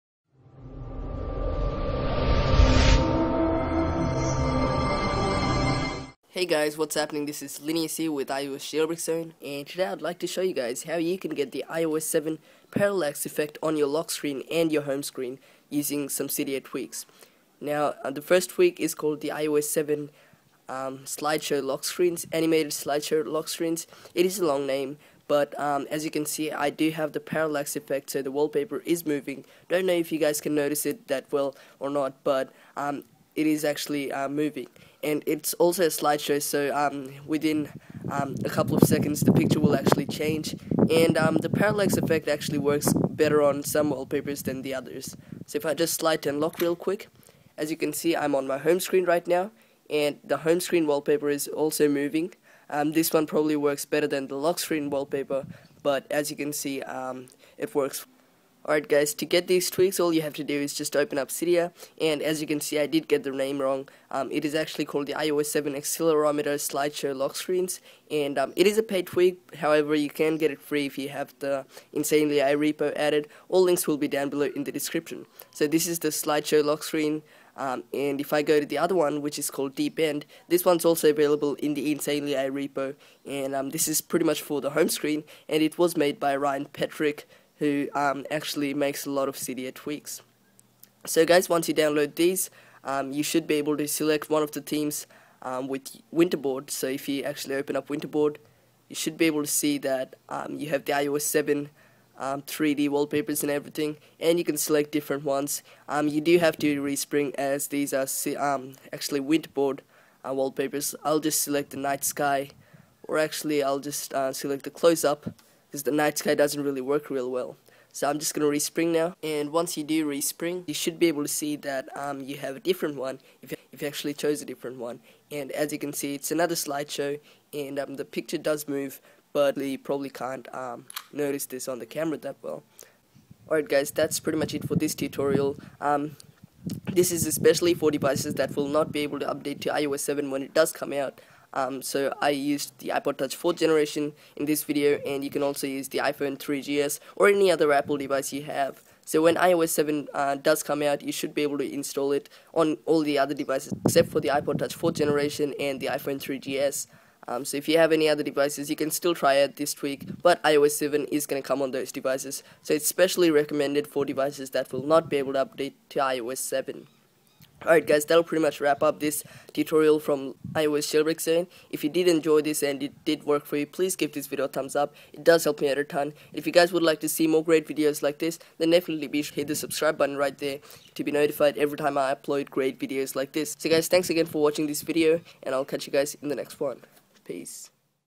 How To Get iOS 7 Parallax Effect On iOS 6 or Below Using Cydia Tweaks!
Hey guys, Linius here and today I will be showing you how you can get the iOS 7 Parallax effect on devices running iOS 6 or even below by simply using two jailbreak tweaks!

Tweaks Used:
- iOS7 Accelerometer Slideshow Lockscreens
- Deepend
Get FREE YouTube Views, Likes and Subs!
Top 20 Cydia Tweaks For iPhone/iPad & iPod Touch of August 2013